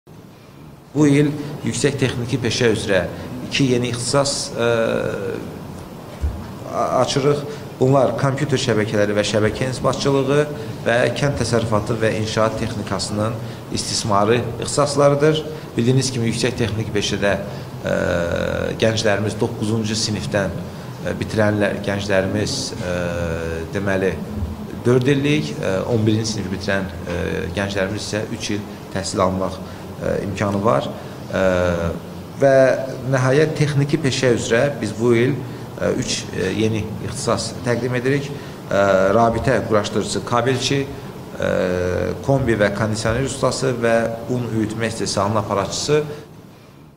Texniki peşə üzrə 2 yeni ixtisas açılır
“Yeni tədris ilindən yüksək texniki peşə sahəsi üzrə iki yeni ixtisas açılacaq. Bunlar kompüter şəbəkələri və şəbəkə inzibatçılığı, kənd təsərrüfatı və inşaat texnikasının istismarı ixtisaslarıdır”.
Bu barədə Peşə Təhsili üzrə Dövlət Agentliyinin direktoru vəzifəsini icra edən Ceyhun Kərəmov məlumat verib.
O həmçinin qeyd edib ki, texniki peşə sahəsi üzrə isə üç yeni ixtisas – rabitə quraşdırıcısı kabeli, kombi və kondisioner ustası, eləcə də unüyütmə istehsalının aparatçısı tədris olunacaq.